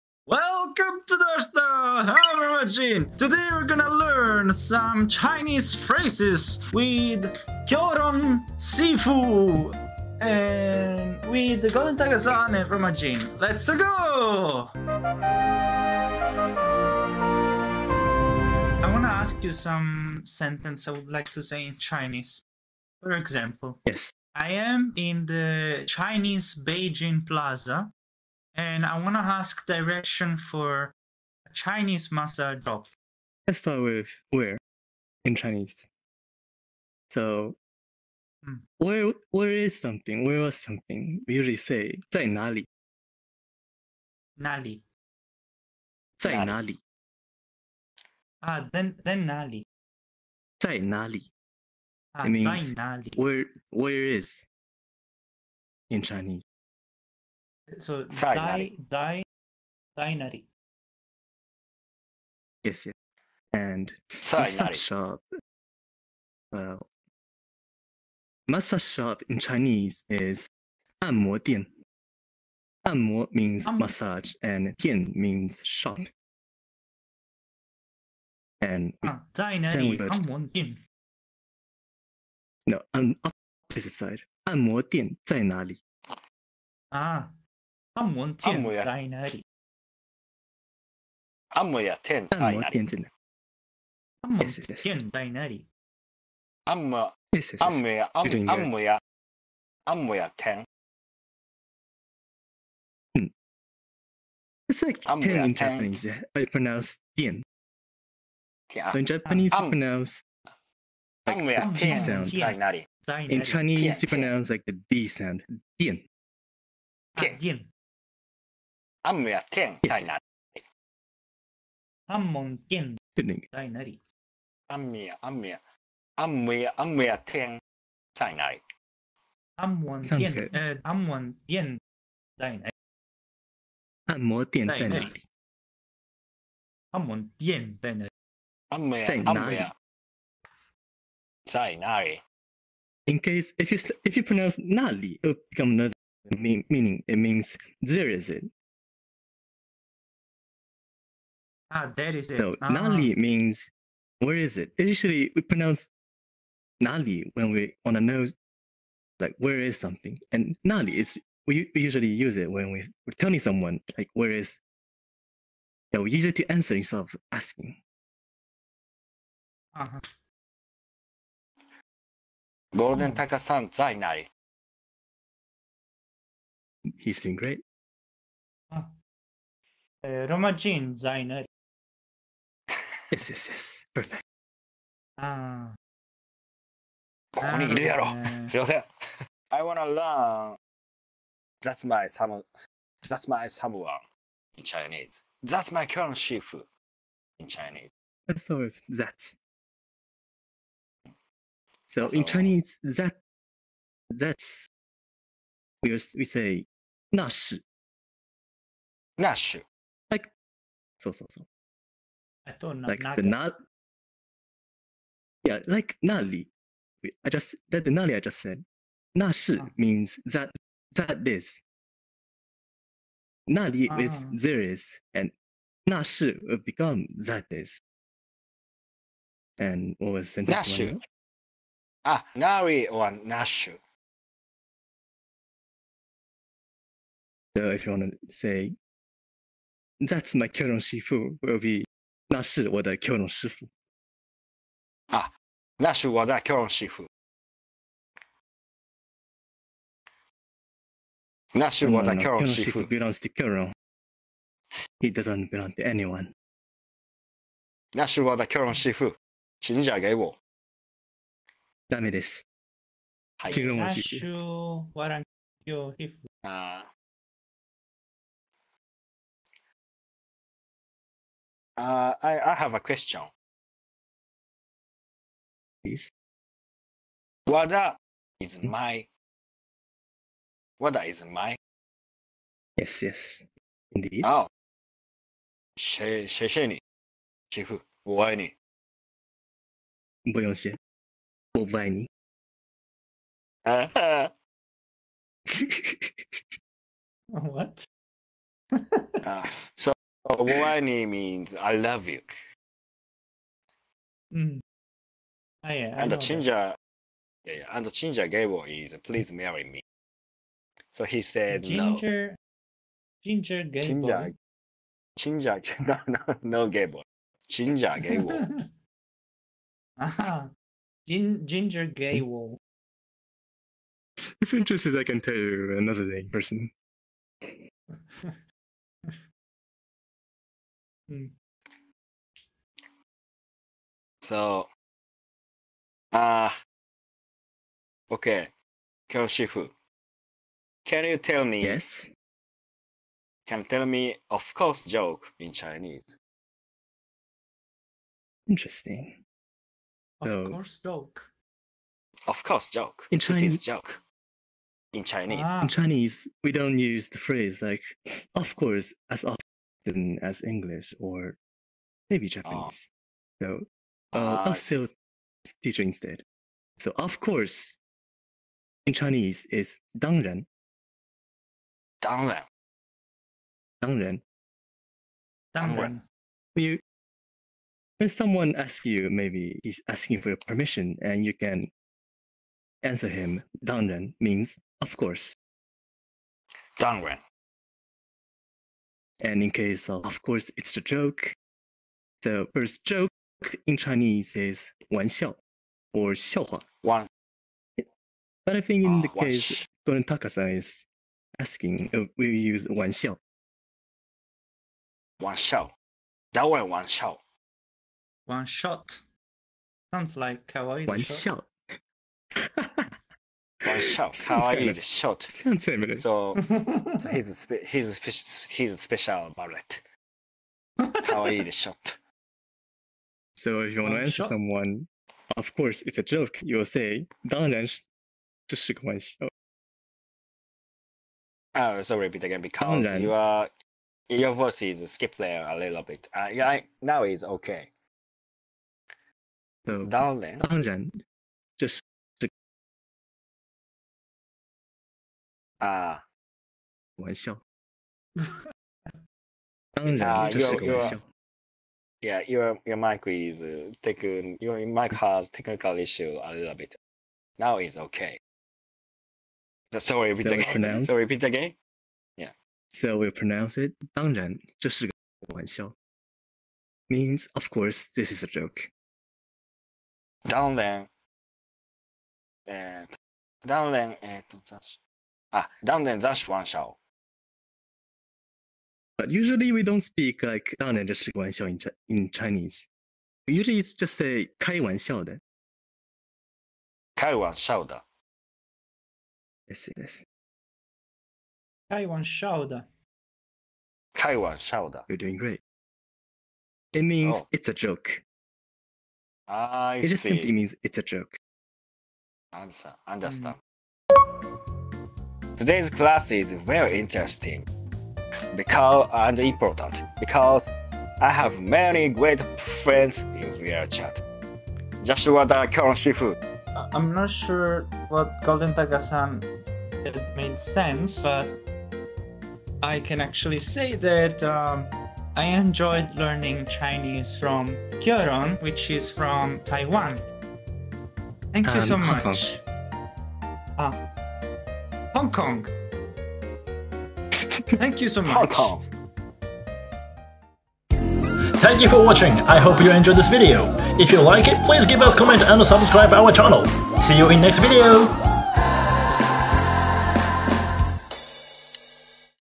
#415 Chinese Lesson in VR vol.1
We tried to learn Chinese in VR.